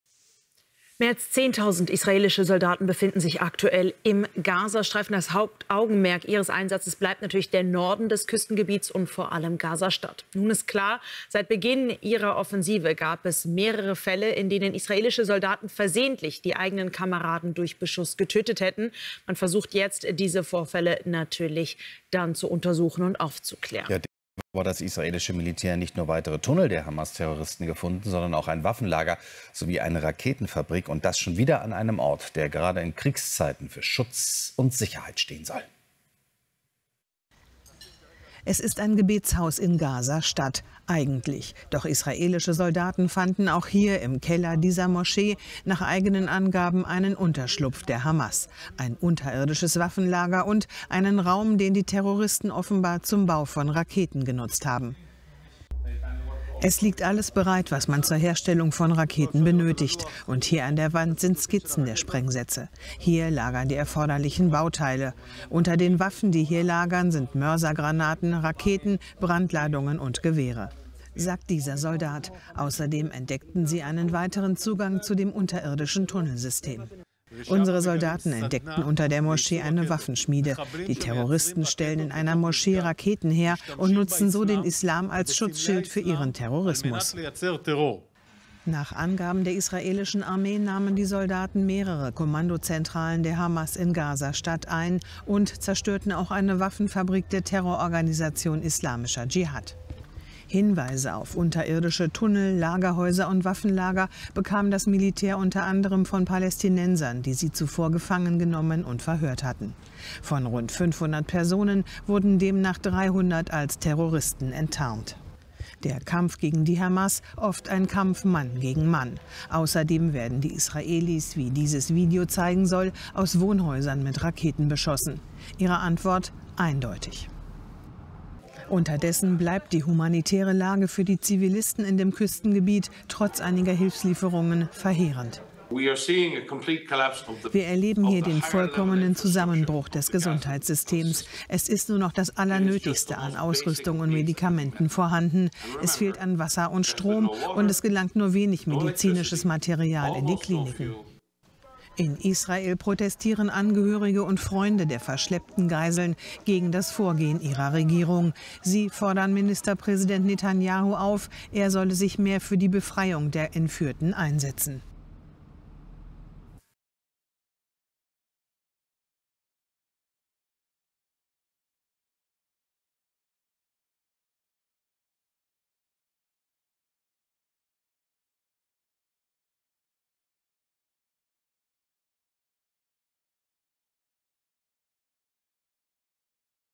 KAMPF UM GAZA: Gnadenlose Gefechte - Israels Soldaten entdecken Raketenfabrik unter Moschee
Die israelische Armee hat eigenen Angaben zufolge innerhalb eines Tages 250 Stellungen der islamistischen Hamas im Gazastreifen aus der Luft angegriffen. Unter den am Montag beschossenen Stellungen seien unter anderem Raketenwerfer und Dutzende Mitglieder der Hamas, teilte das Militär am Dienstag mit. Zudem sei in der Nacht auf Dienstag ein Posten beschossen worden, von dem aus am Montag Raketen auf das Zentrum Israels abgefeuert wurden. Der «Raketenabschusspunkt» befand sich den Angaben nach in der Nähe eines Wohngebiets.

Extremistische Palästinenser im Gazastreifen feuerten auch in der Nacht auf Dienstag wieder Raketen Richtung Israel ab. In einem israelischen Ort in der Grenzregion des Gazastreifens wurde laut Militär Raketenalarm ausgelöst. Seit Beginn des Kriegs am 7. Oktober sind israelischen Armeeangaben zufolge Tausende Raketen aus dem Gazastreifen auf Israel abgeschossen worden. Seit der Bodenoffensive sei die Zahl der Abschüsse aber deutlich zurückgegangen.